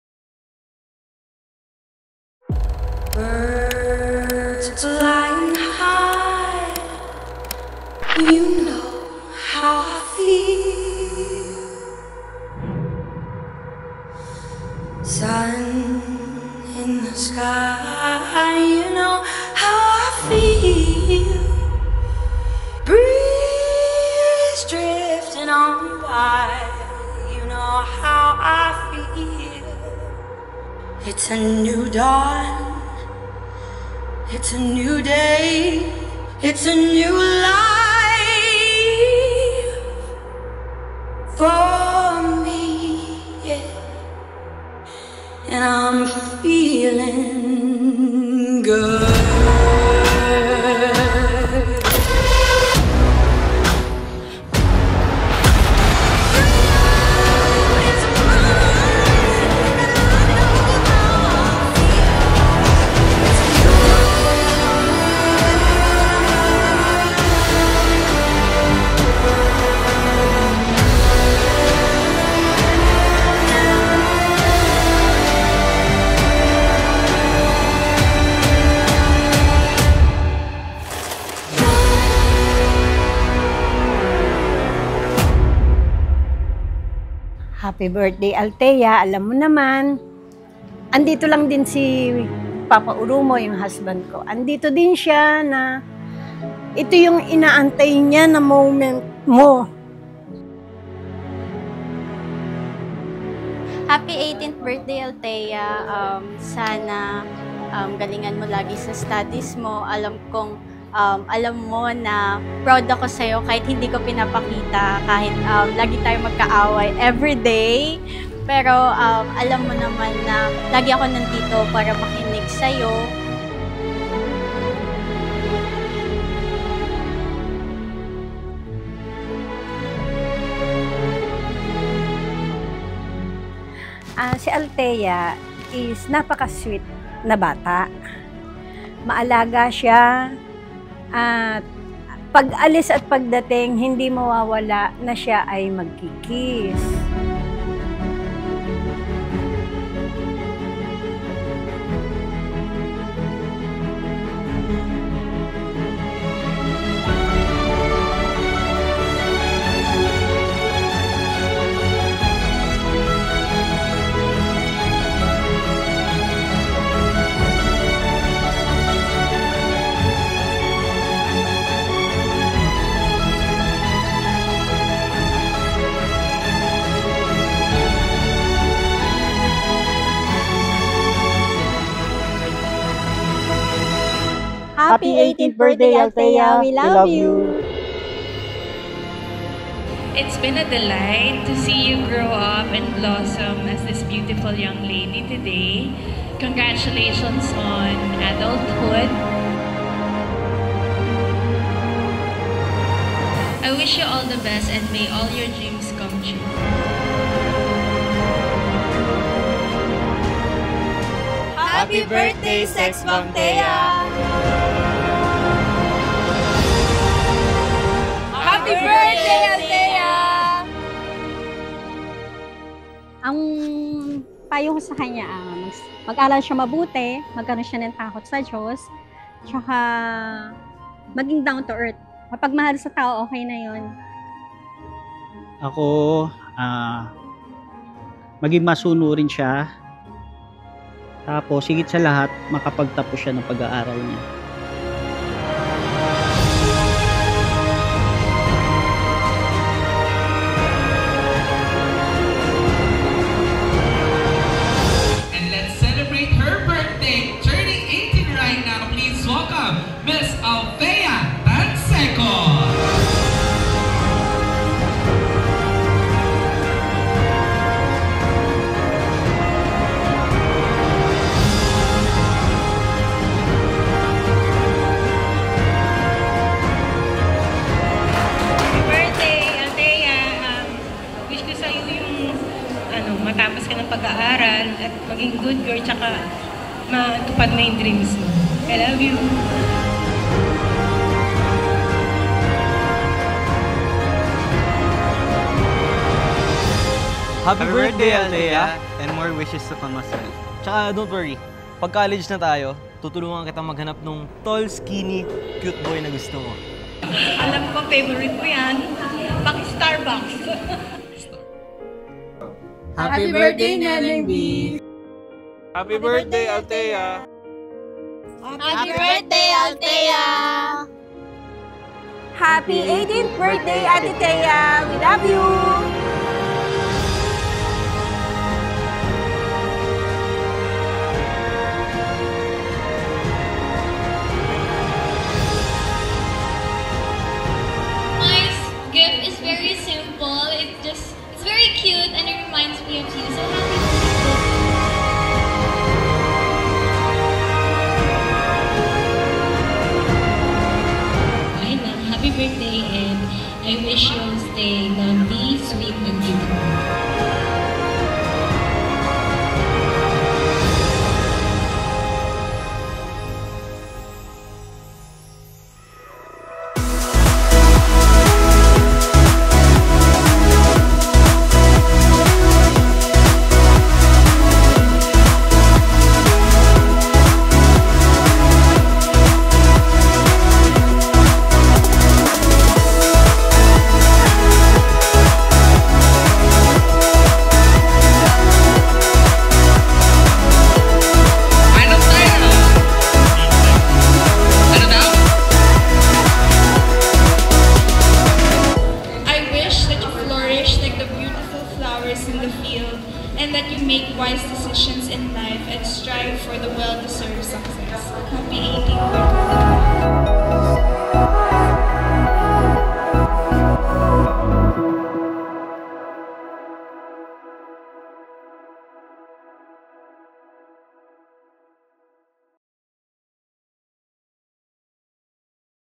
Althea @18 | Same Day Edit Video
Althea @18 | Same Day Edit Video | Scarlet Package
Theme: Bridgerton
Full Event Coordination | Photo/Video Coverage | Same Day Edit | Professional Singing Host | Photo Booth